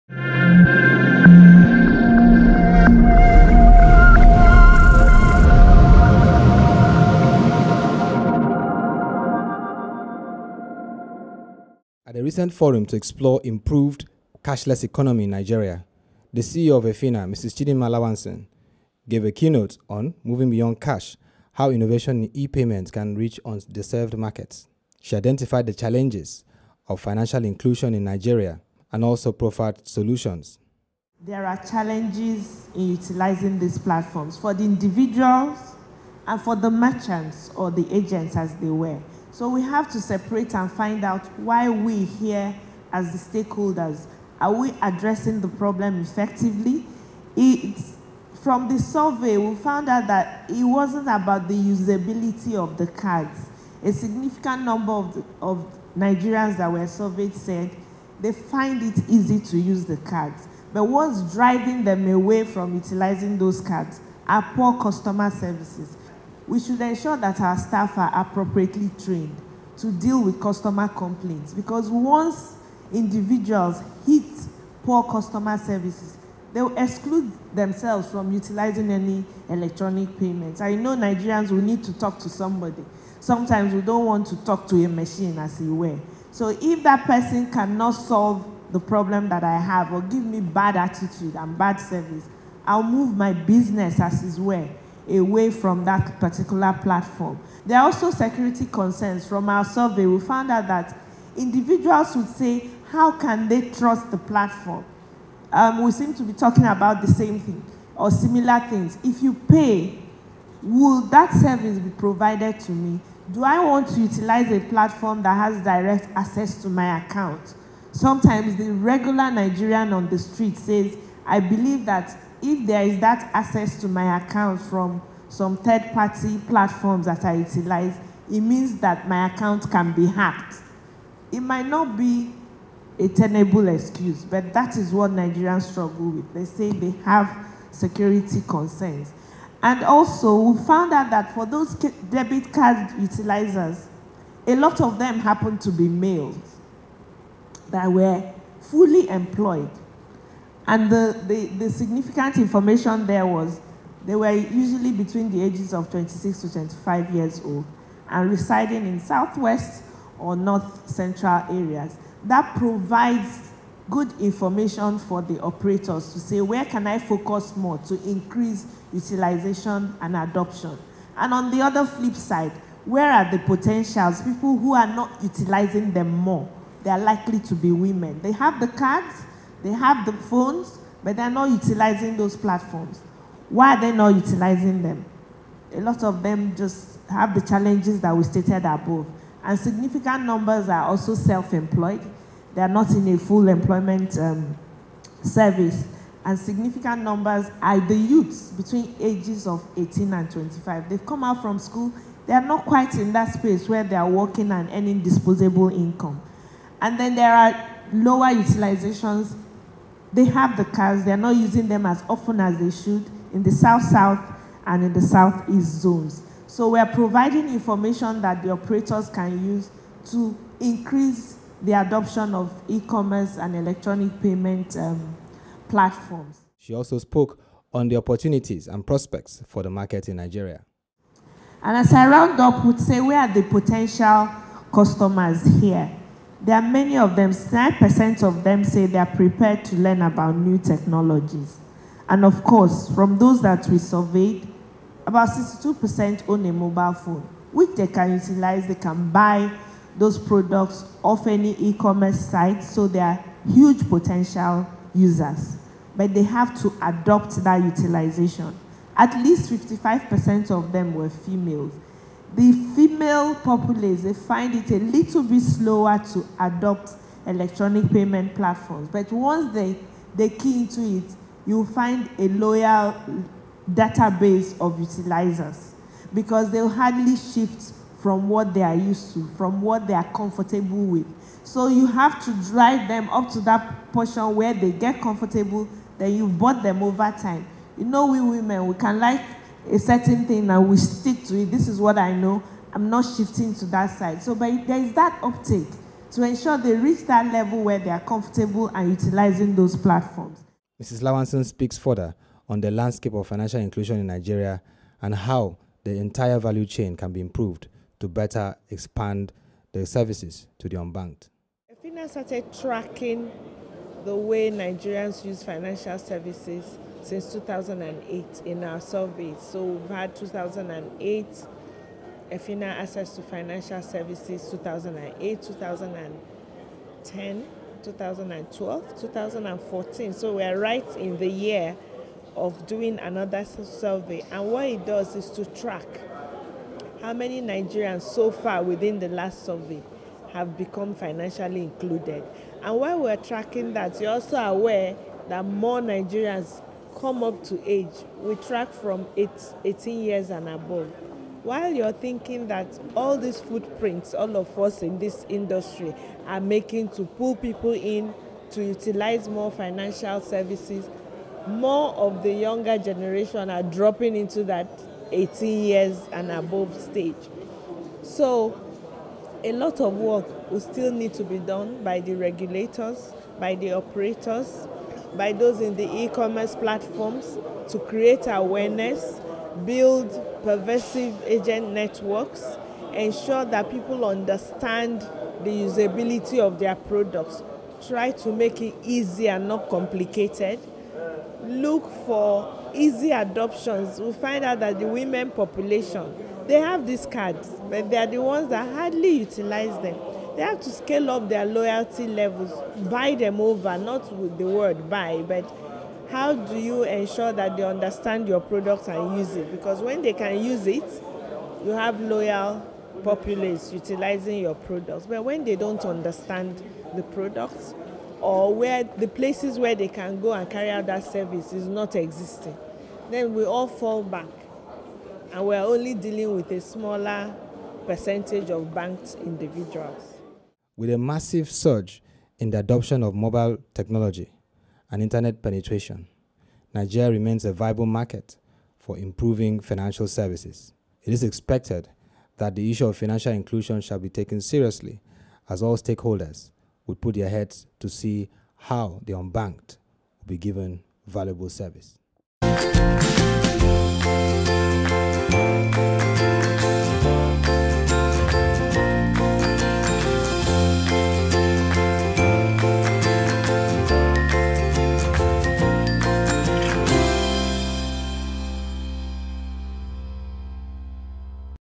Financial Inclusion in Nigeria:Challenges & Prospects
Financial inclusion is critical to Nigeria and Africa, as we see a rapid surge in mobile penetration/ adoption and use of smartphones/devices, which is a platform for improving financial services, even to the unbanked in the rural areas.

The CEO of EFiNA(Enhancing Financial Innovation & Access) Mrs Chidinma Lawanson recently gave a presentation on how the landscape in Nigeria, can experience transformation.

Proshare WebTV brings excerpts